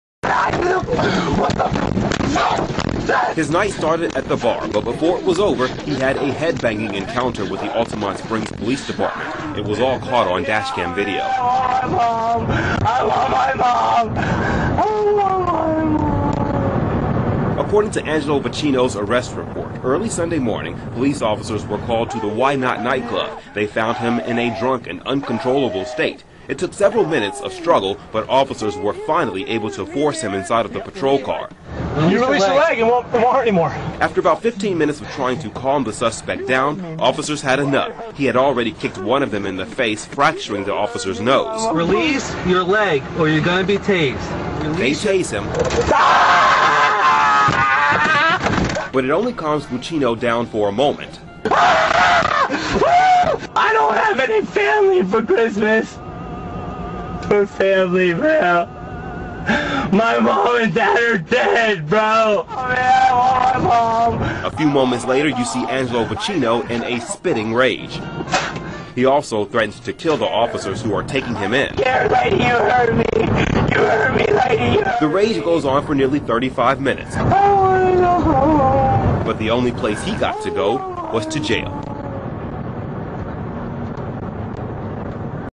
Man Vs Cop Car - Bath Salts Rampage Inside of Police Car
bath salt